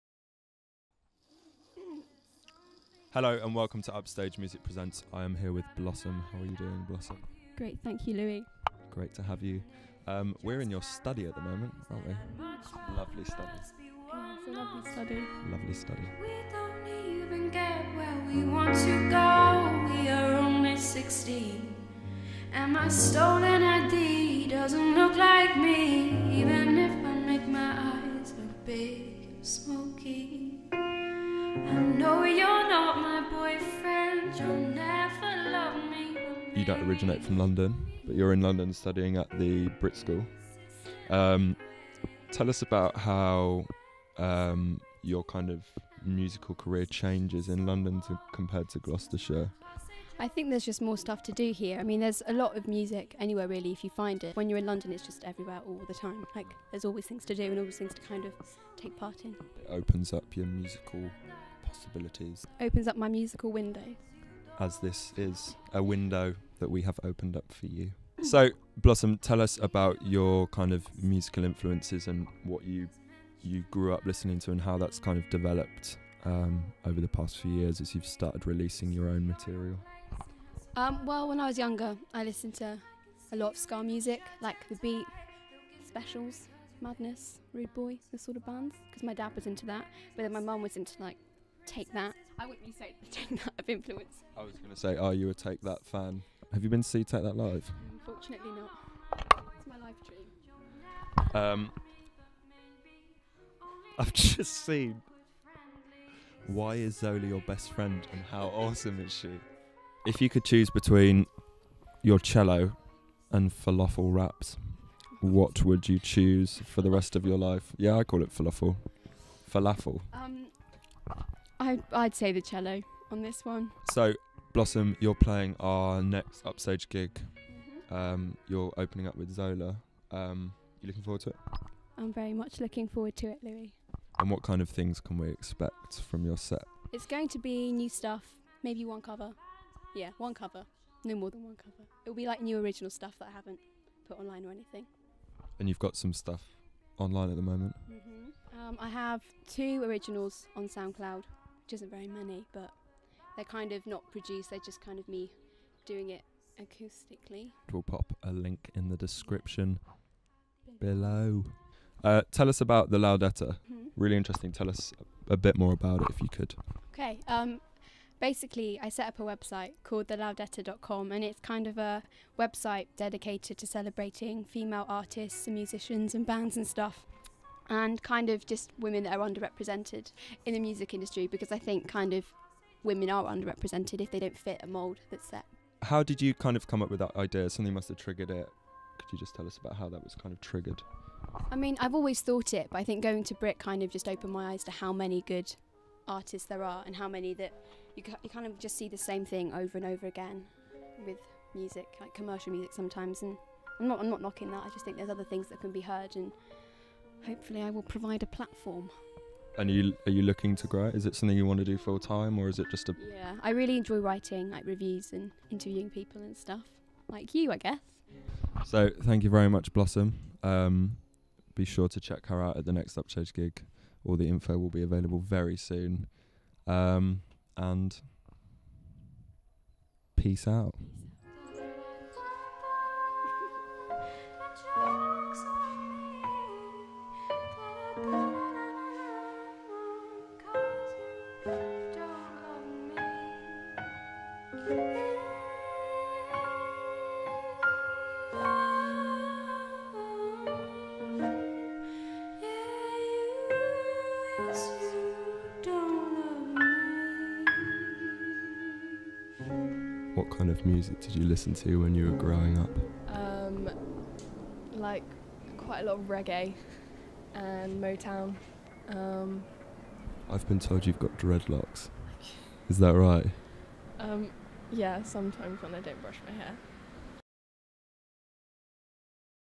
UPSTAGE Presents: Blossom Caldarone
UPSTAGE sit down with Blossom Caldarone, a 17 year old musician from Gloucestershire, on a rare sunny day in London. We invite you to join us as we get a peek (through a window) into her life, her love of her cello and her thoughts on female artists in music.

Directed by George O'Regan

Filmed by George O’Regan, Clint Frift and Theo Batterham

Edited by Theo Batterham

Interview by Louis Suggett